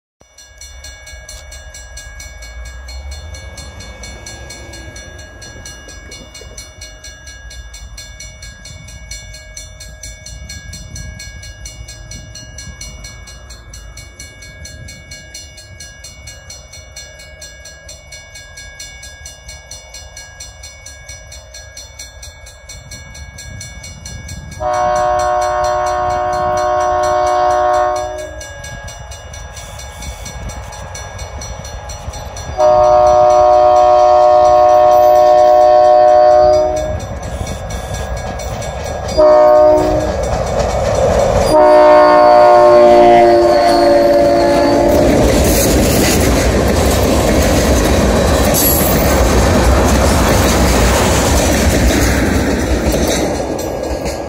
IMRR 61 east
the IMRR SD18 twins with a short 4 car TPW east bound road train east of Eureka, IL 12-22-17